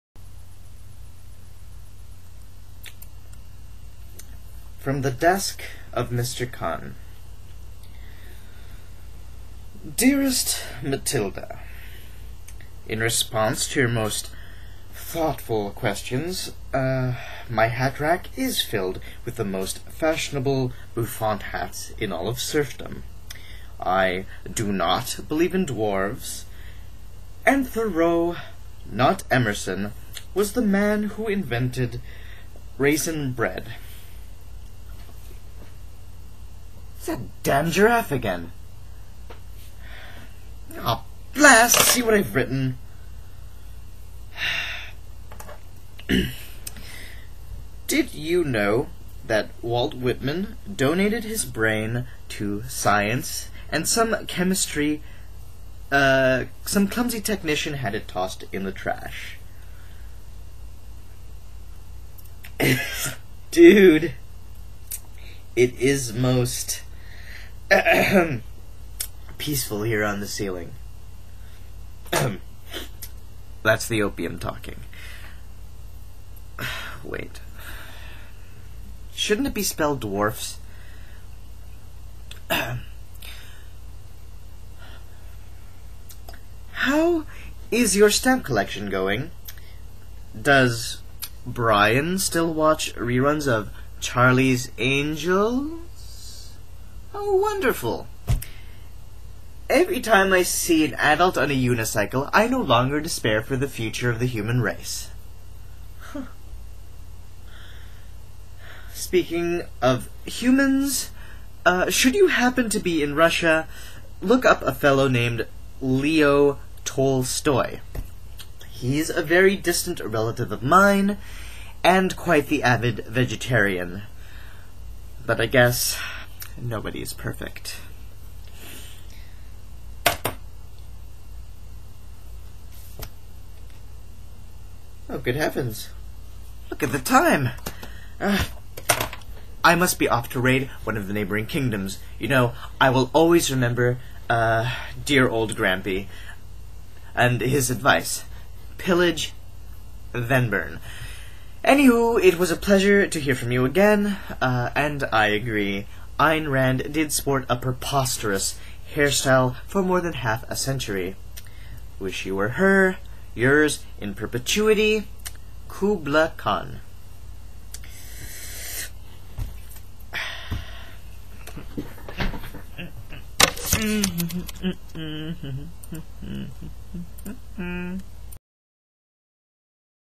From the Desk of Mr Khan by Greg Santos
Greg Santos reads the poem "From The Desk of Mr. Khan"